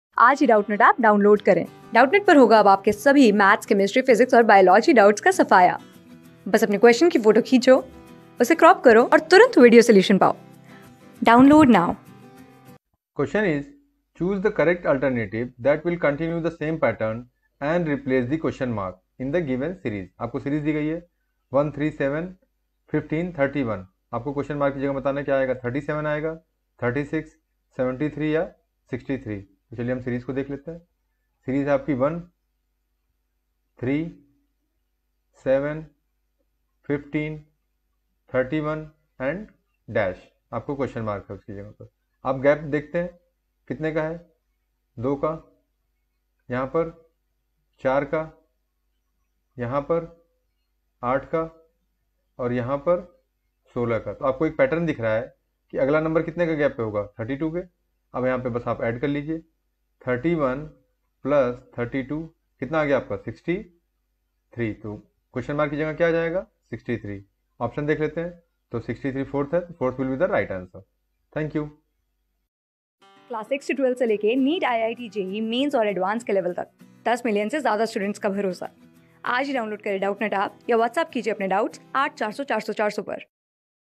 Choose the correct alternative that will continuethe same pattern and replace the questions mark...
Choose the correct alternative that will continuethe same pattern and replace the questions mark (?) in the given series 1, 3, 7, 15, 31, ?
Class: 14
Subject: REASONING
Chapter: NUMBER SERIES
Board:GOVT

👉 Have Any Query? Ask Us.